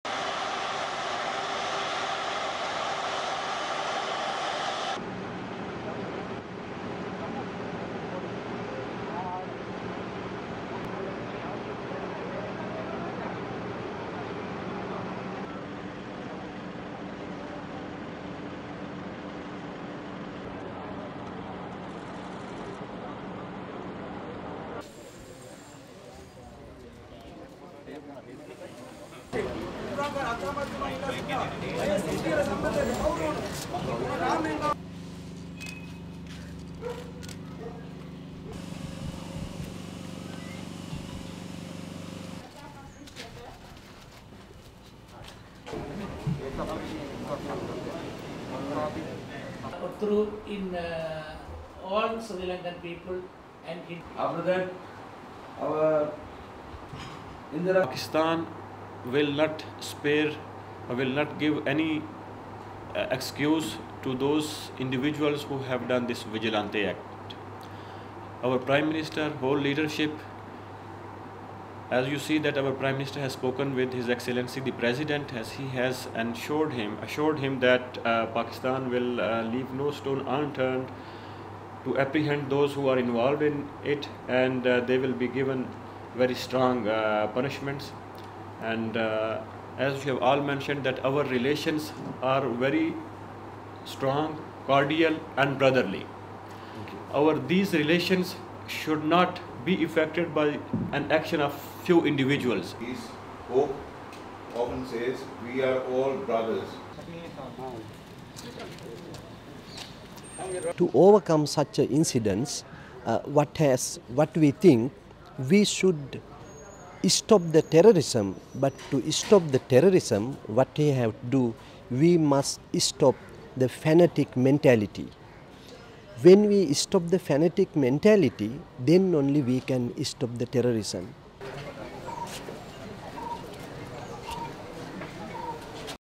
Remains of lynched Sri Lankan arrives from Pakistan
(6 Dec 2021) The charred remains of a factory manager who was lynched by a mob in Pakistan over alleged blasphemy was brought back to Sri Lanka on Monday.
Sri Lankan national Priyantha Kumara was assaulted by a mob of hundreds of people before being dragged onto the streets and set on fire last Friday in Sialkot, Pakistan, where he helped run a sports equipment factory.
Workers at the factory accused him of desecrating posters bearing the name of Islam's Prophet Muhammad.
Sir Lankan government officials received Kumara's remains in a wooden box decorated by flower garlands, before preparations to hand over the coffin to his family to perform his last rites.
Hours before the arrival of the remains, dozens of activist and religious groups gathered before Pakistan's mission in Sri Lanka's capital of Colombo demanding justice for the victim.
“Pakistan will leave no stone unturned to apprehend those involved. They will be given very strong punishments," Pakistan's acting ambassador to Sri Lanka, Tanvir Ahmad, said.
He was speaking with Buddhist, Hindu, Muslim and Christian leaders who met him at the Pakistani mission.
Ahmad said that Pakistan's prime minister had spoken with Sri Lankan President Gotabaya Rajapaksa to reassure him that Pakistani authorities were investigating the lynching.
Prime Minister Imran Khan in a phone call with Rajapaksa also said Pakistani police had arrested more than 100 people in connection with the killing.
In the conservative society of Pakistan, mere allegations of blasphemy can trigger mob attacks.
The country’s blasphemy law carries the death penalty for anyone found guilty of the offense.